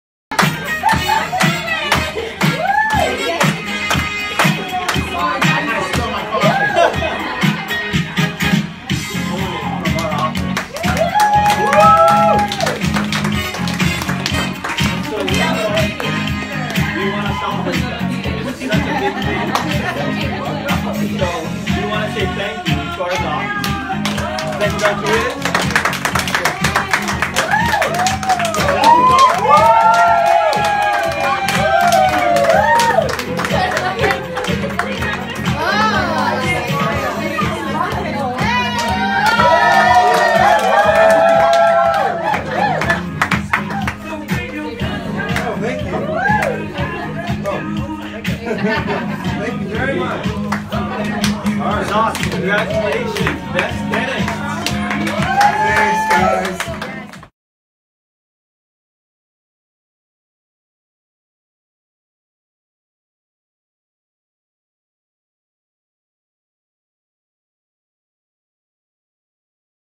Kona's Best Dentists!
We had 7 of the dentist at our location get Best Dentists! A big congratulations to you guys! The Kona team are and always will be grateful for all your guys hard work and dedication to your patients!